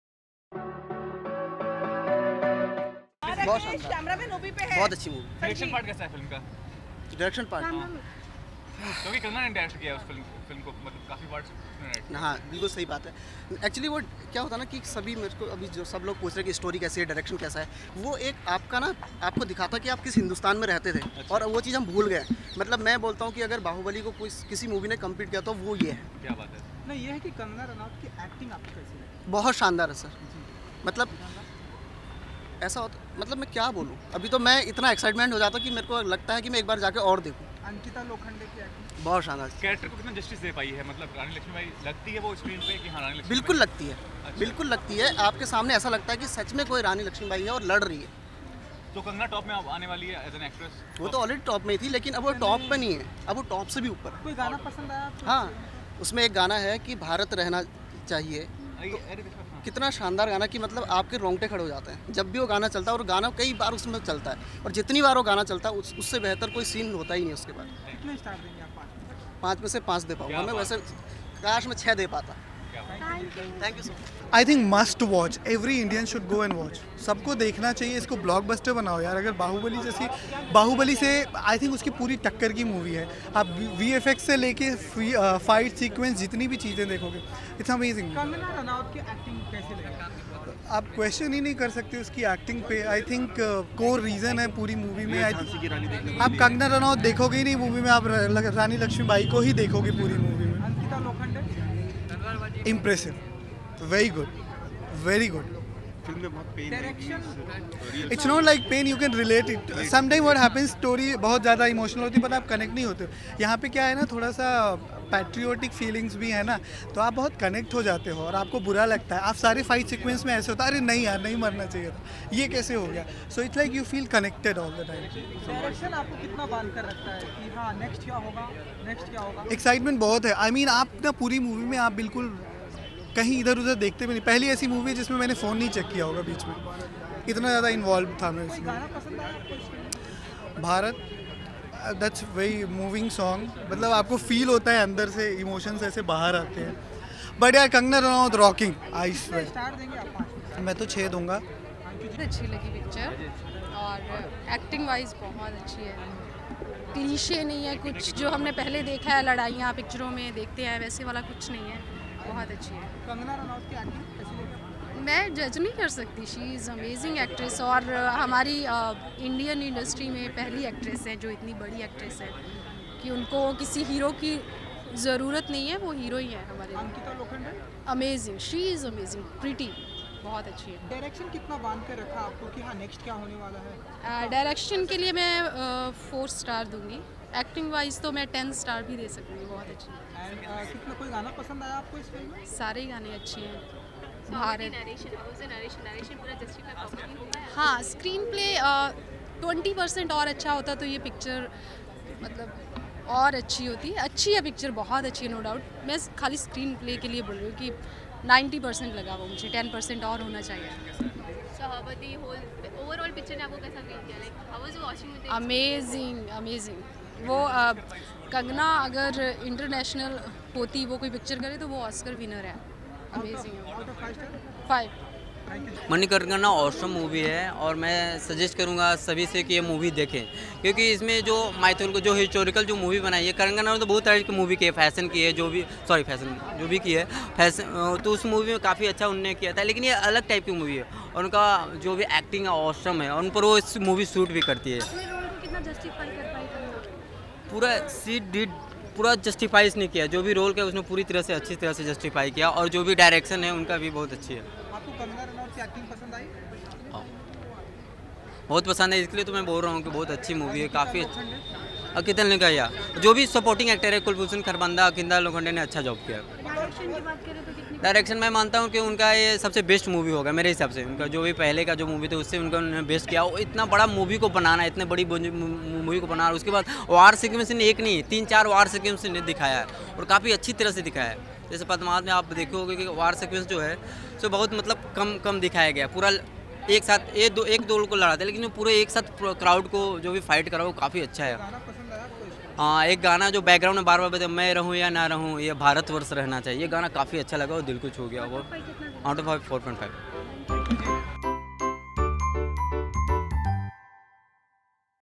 Manikarnika Movie CRAZY Public Review | Kangana Ranaut , Ankita Lokhande
Watch the Public Review Of Kangana Ranaut's latest release film 'Manikarnika'.